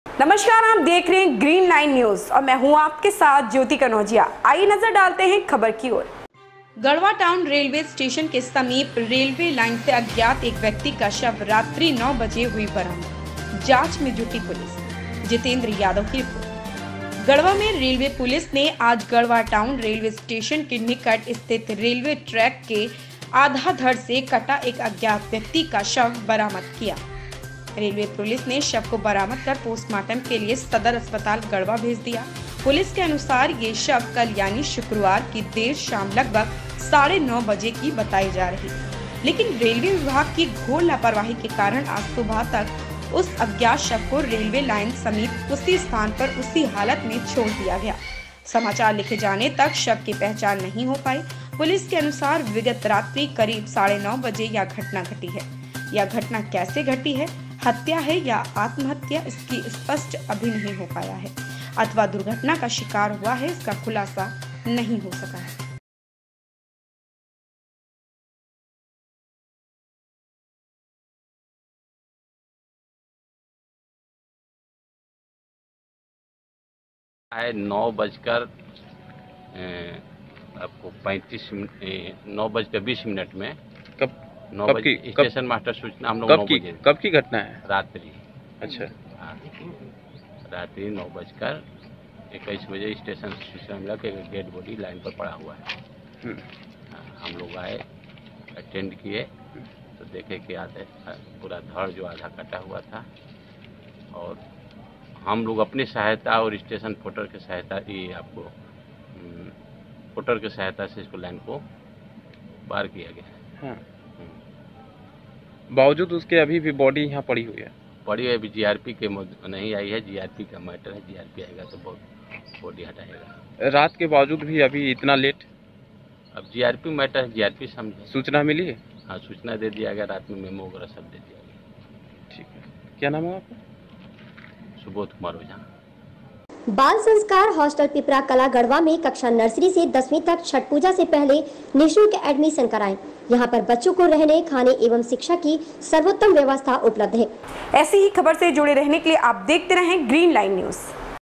रेलवे स्टेशन के समीप रेलवे लाइन से अज्ञात एक व्यक्ति का शव रात्रि 9 बजे हुई बरामद,
More information---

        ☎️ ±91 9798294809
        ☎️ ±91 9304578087